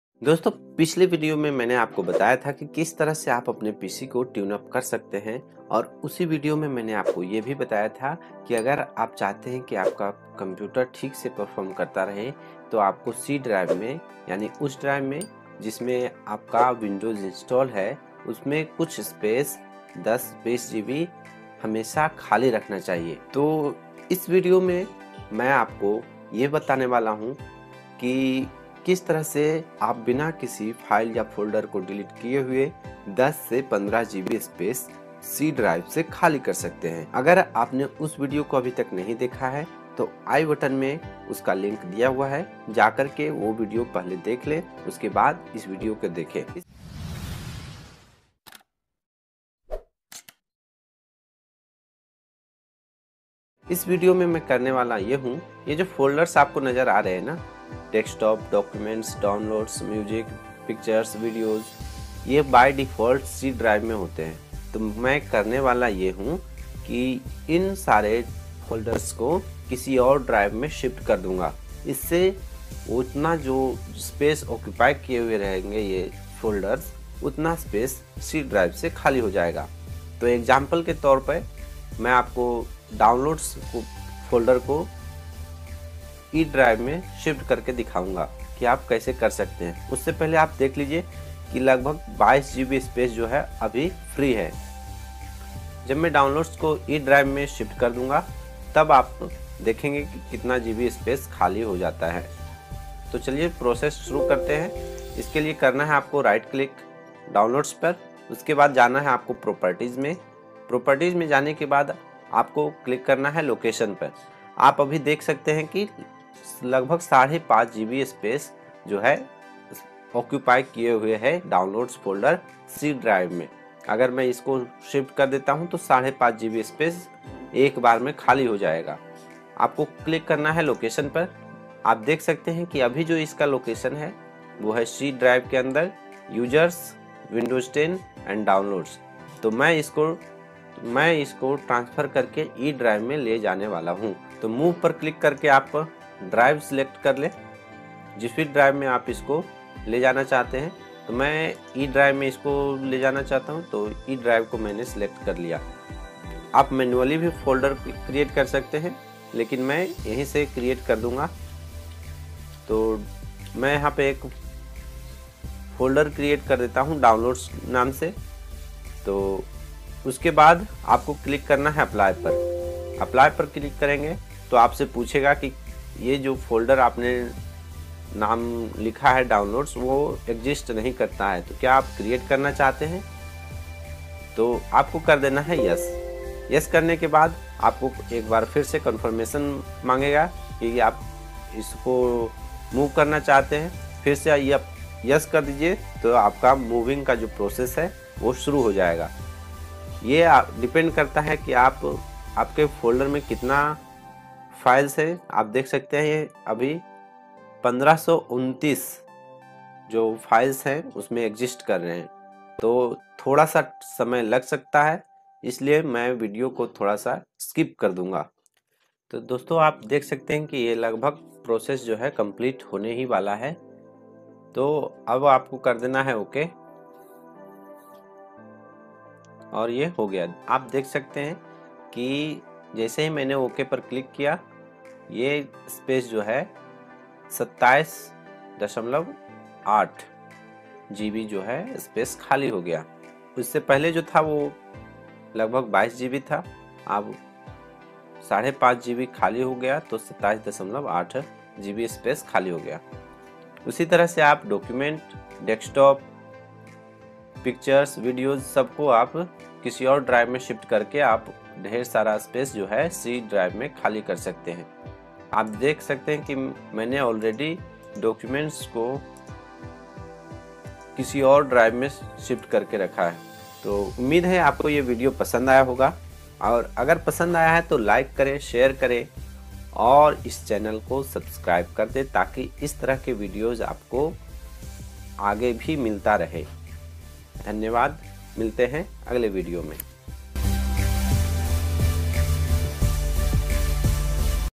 How To Clean C Drive In Windows 10 | How To Free Up 20GB Space In C Drive | Make Your PC Faster
Dosto Es Video Me Hamne bataya Hai ki Kaise Aap C Drive 20 GB Space Free Kar Sakte Hain.